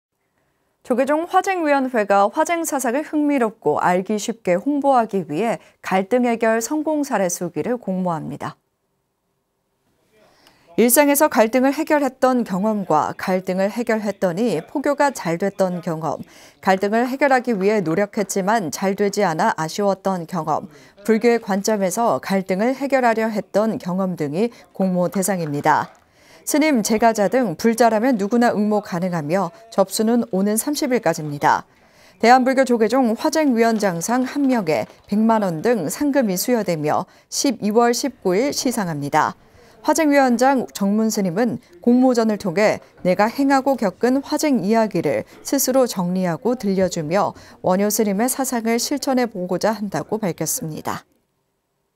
[BTN뉴스] 조계종 화쟁위, ‘갈등해결 성공사례 수기’ 공모
조계종 화쟁위원회가 화쟁 사상을 흥미롭고 알기 쉽게 홍보하기 위해 '갈등해결 성공사례 수기'를 공모합니다.

일상에서 갈등을 해결했던 경험과 갈등을 해결했더니 포교가 잘 됐던 경험, 갈등을 해결하기 위해 노력했지만 잘 되지 않아 아쉬웠던 경험, 불교의 관점에서 갈등을 해결하려 했던 경험 등이 공모 대상입니다.

스님, 재가자 등 불자라면 누구나 응모 가능하며 접수는 오는 30일까지입니다.

대한불교조계종 화쟁위원장상 1명에 100만원 등 상금이 수여되며 12월 19일 시상합니다.

화쟁위원장 정문스님은 “공모전을 통해 내가 행하고 겪은 화쟁이야기를 스스로 정리하고 들려주며 원효스님의 사상을 실천해 보고자 한다"고 밝혔습니다.